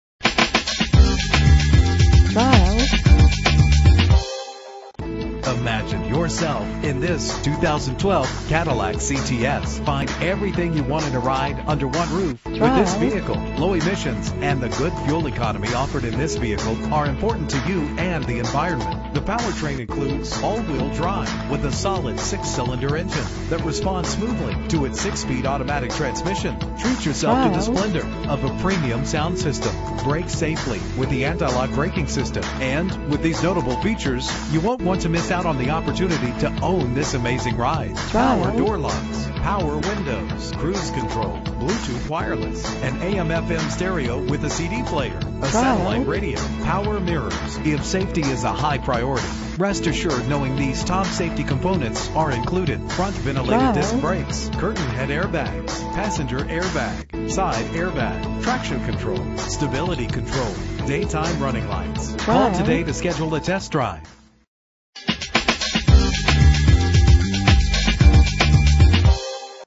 2012 Cadillac CTS - Dearborn MI
Year: 2012
Make: Cadillac
Model: CTS
Trim: Base
Engine: 3.0 liter 6 cylinder 24 valve
Transmission: 6-Speed Automatic
Color: Black Raven
Mileage: 0
Address:
21730 Michigan